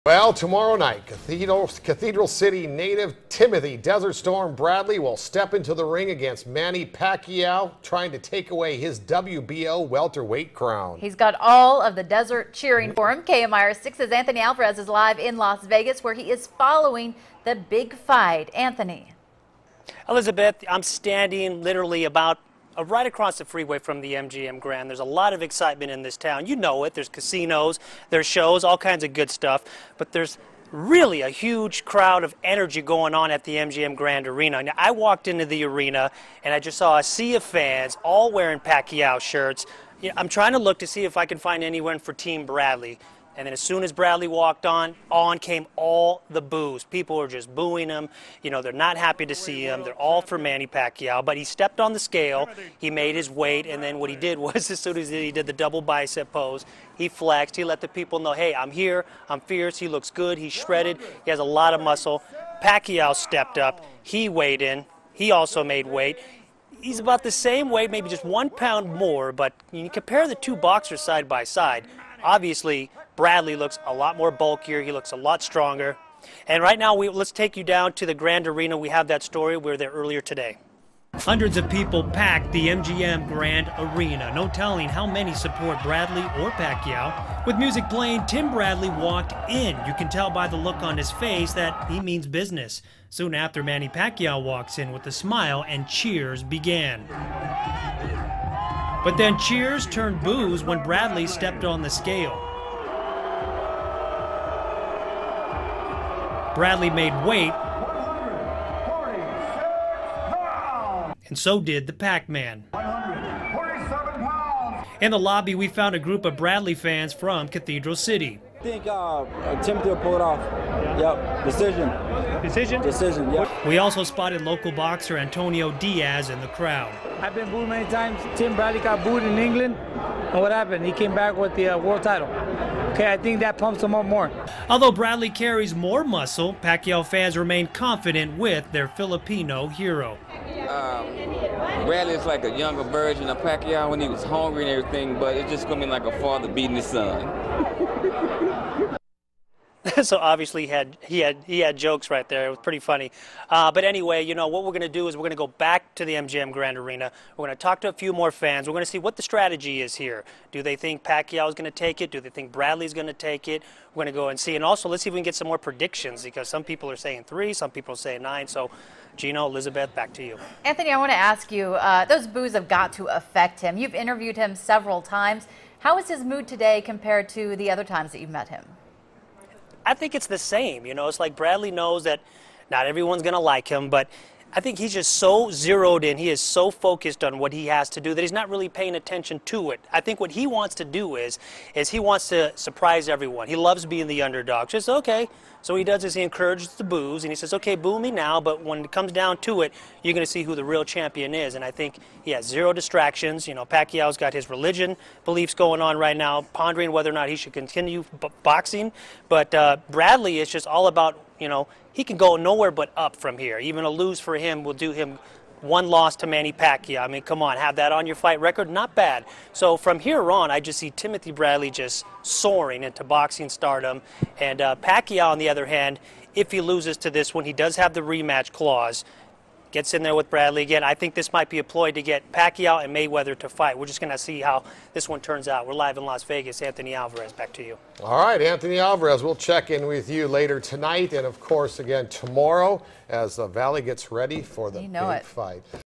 Timothy Bradley Faces Cheers and Some Jeers In Vegas
Local boxing star Timothy Bradley weighed in today in Las Vegas, ready to face Manny Pacquiao in the ring Saturday night at the MGM Grand. Bradley goes into the fight as the underdog, but he remains optimistic about his odds of beating The Pacman.